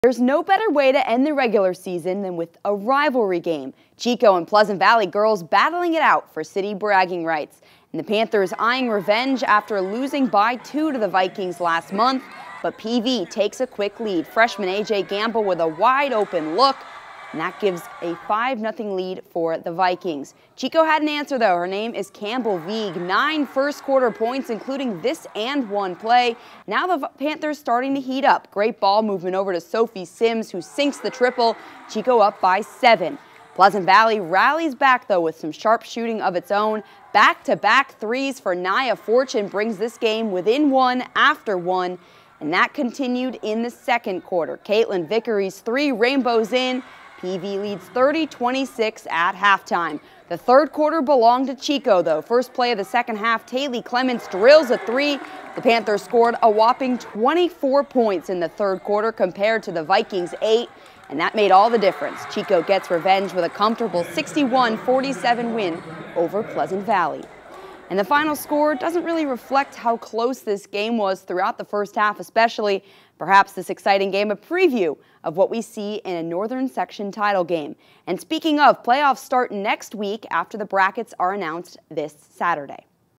Chico Girls Basketball gets revenge on Pleasant Valley thanks to a 24-point third quarter
Chico Girls Basketball scored 24-points in the third quarter en route to a 61-47 revenge win over Pleasant Valley. Chico Junior Campbell Vieg led the way with a game-high 26 points.